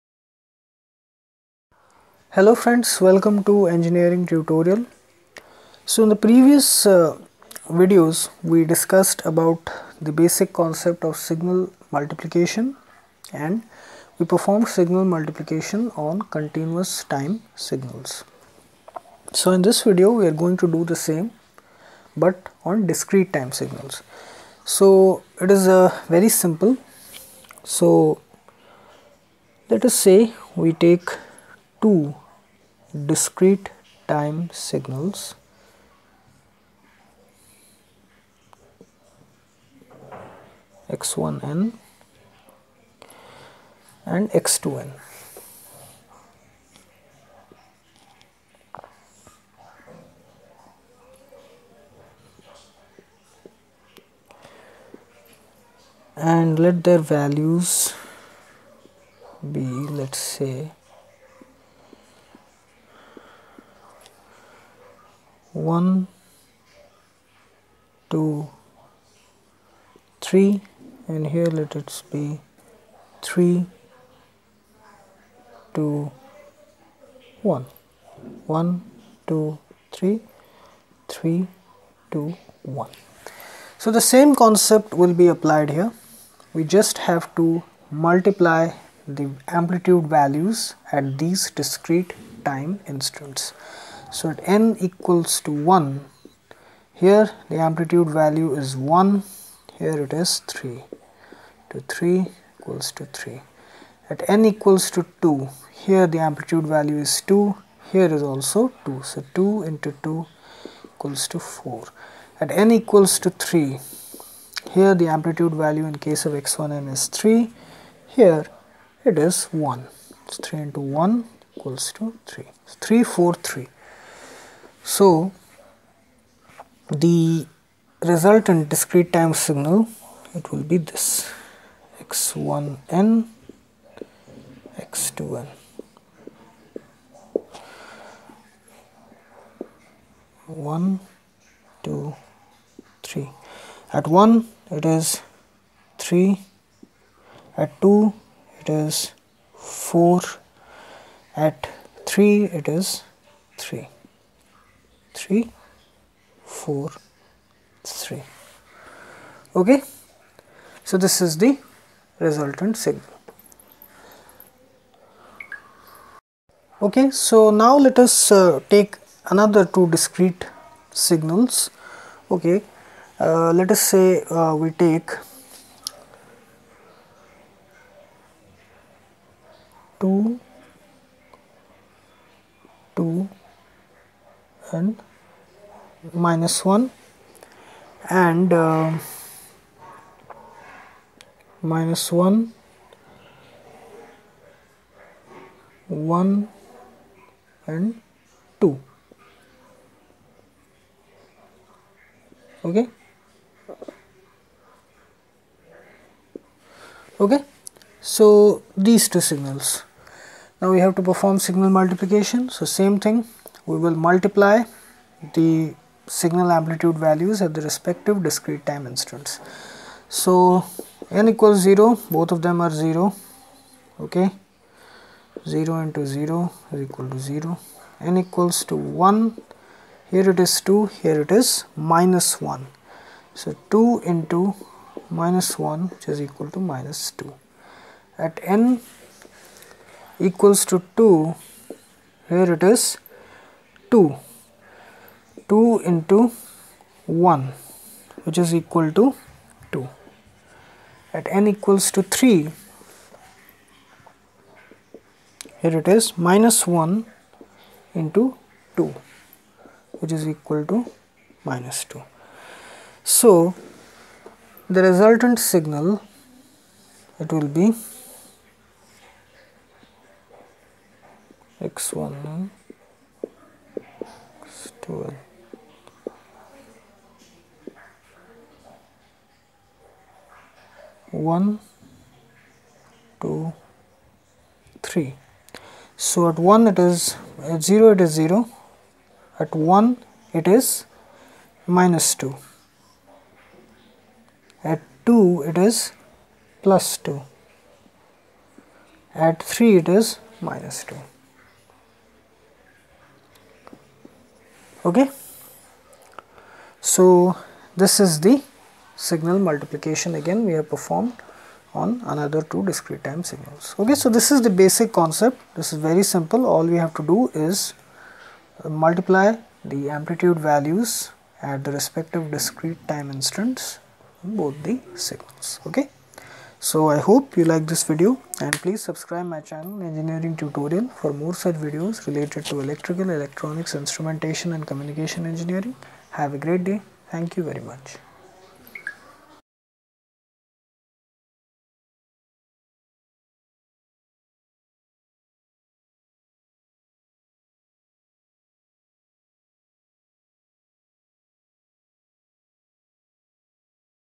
Signal Multiplication | Discrete Time Signals | Basic Concept | Signals And Systems
In this video, we are going to discuss signal multiplication on discrete time signals.